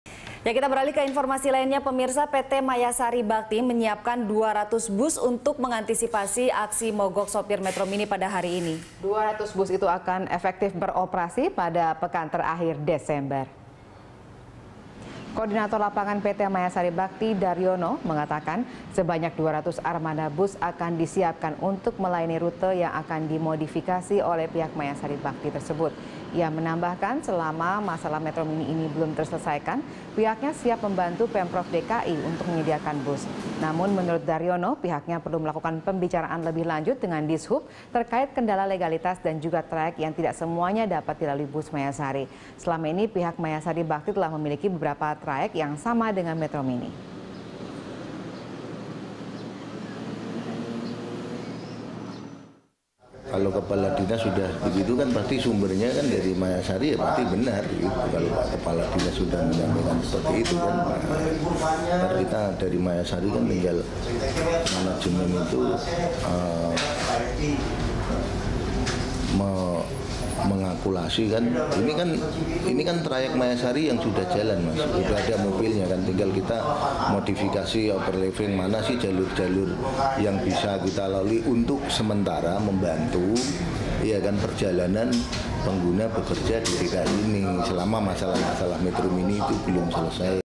PT Mayasari Bakti Siapkan 200 Bus Gantikan Metromini
PT Mayasari Bakti menyiapkan 200 bus untuk mengantisipasi aksi mogok sopir Metromini. 200 bus itu akan efektif beroperasi pada pekan terakhir Desember.

Facebook.com/BeritaSatuTV
Youtube.com/BeritaSatu